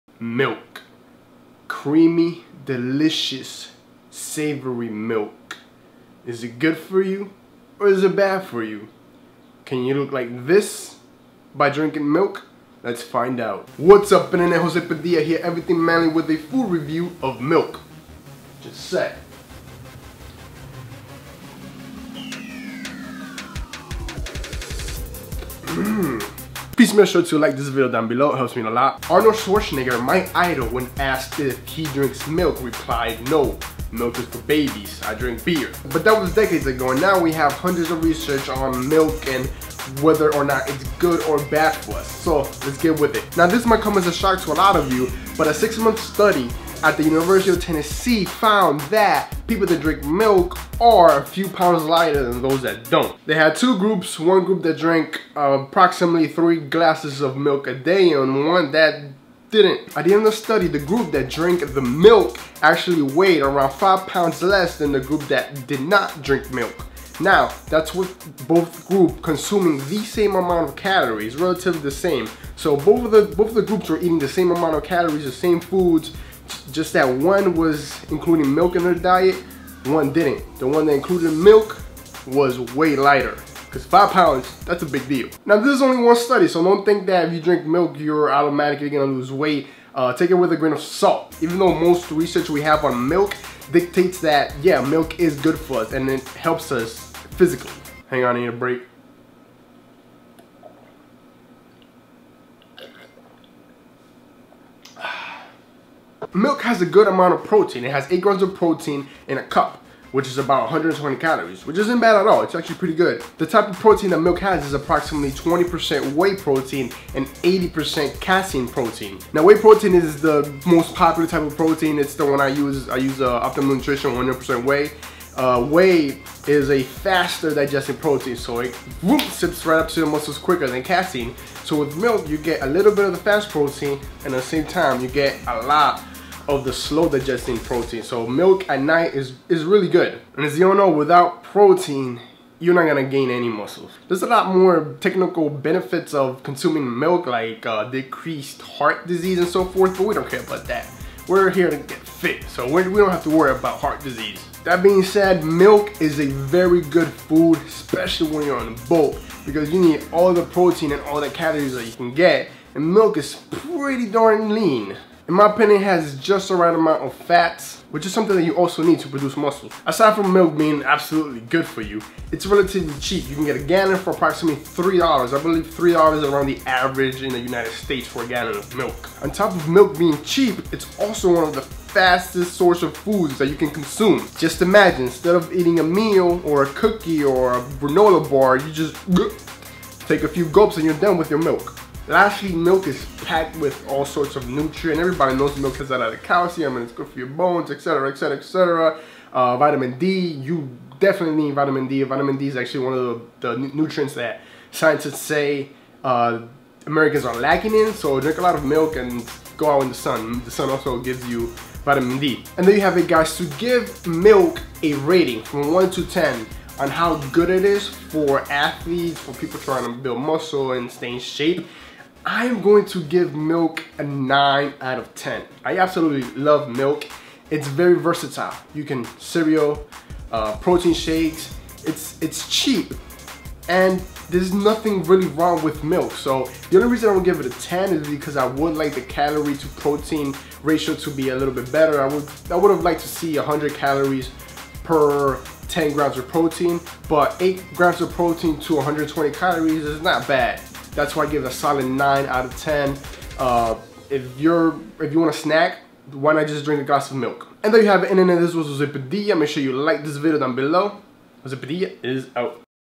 Build Muscles With Milk : Milk Review
Drink milk to lose weight and gain muscles. Want abs ? This study on milk and fat burning will shock you. Use milk to get six pack abs